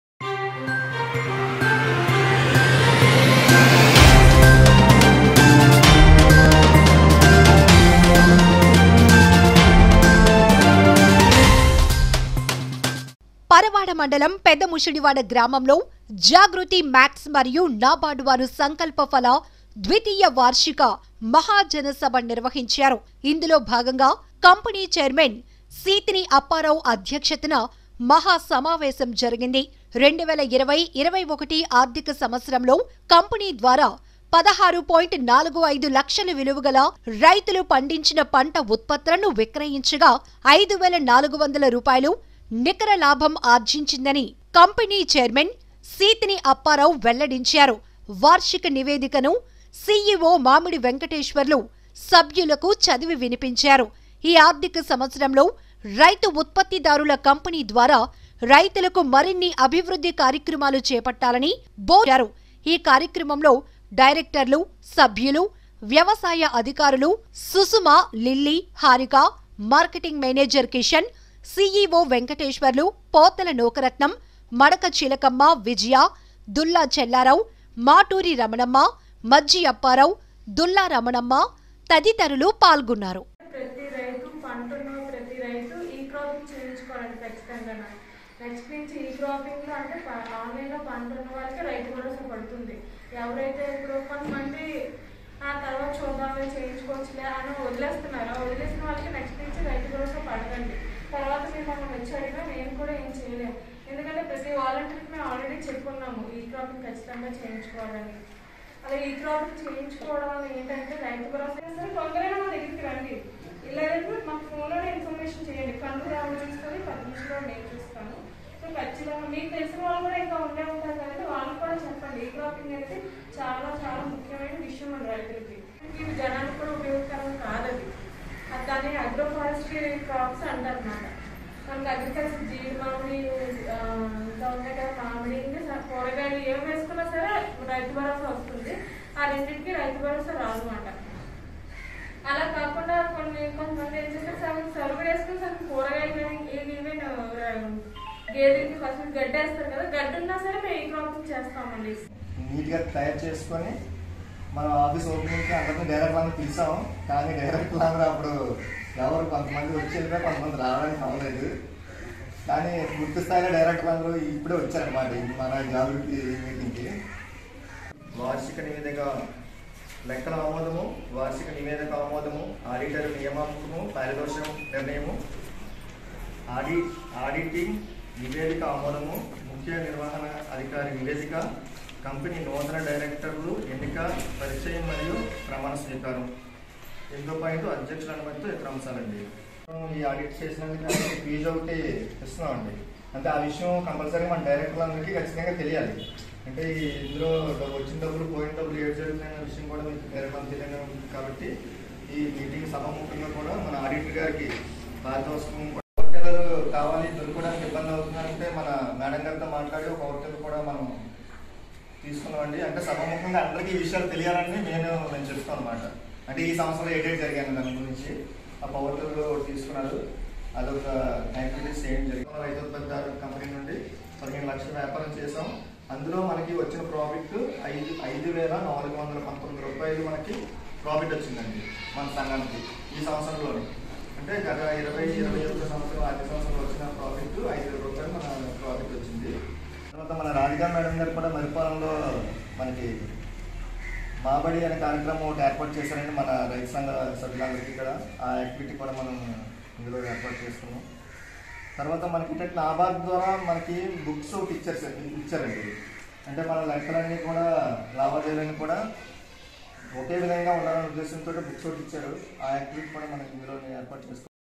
పరవాడ మండలం పెదముసిడివాడలో జాగృతి మాక్ట్స్ మరియు నాబార్డ్ వారు సంకల్పఫల ద్వితీయ వార్షిక మహాజనసభ...
Mee7 News